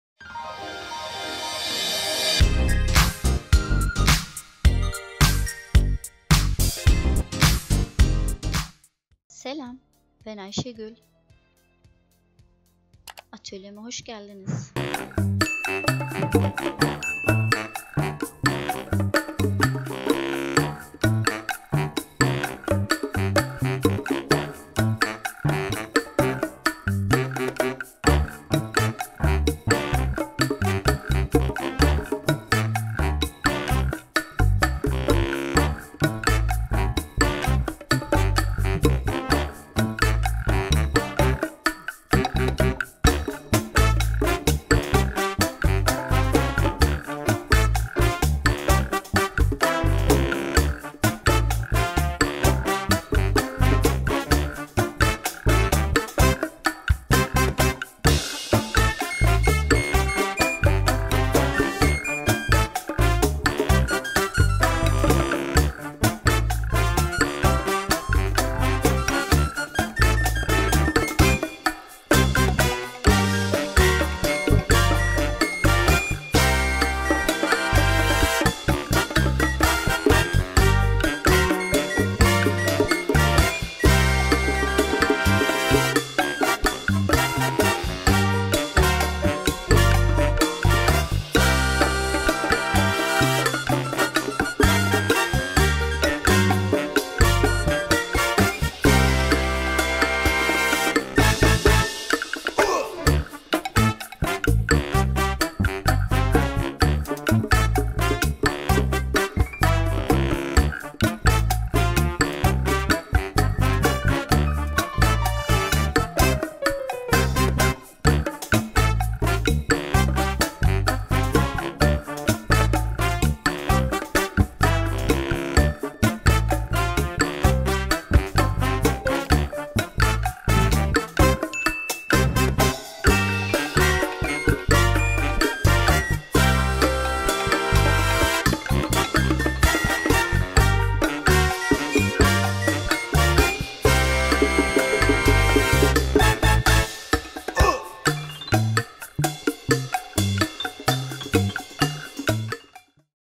DIY | MODERN NİŞAN BOHÇASI SÜSLEMESİ ( Gelin Bohçası Çiçeği, Bavul Süslemesi )
Merhaba. Atölyeme hoş geldiniz 🖐Bugün sizlere, nişan bohça süslemesi nasıl yapılır, bohça için bavul süslemesi için, kurdeleden çiçek nasıl yapılır, kurdele katlama sanatı (kanzashi) tekniği ile bir süsleme, kendin yap,  geri dönüşüm fikri ile geldim. Bu videodan sonra kendiniz yapabilirsiniz ( you can do it yourself after this video)
Umarım yararlı bir video olur 🤗🍀❤️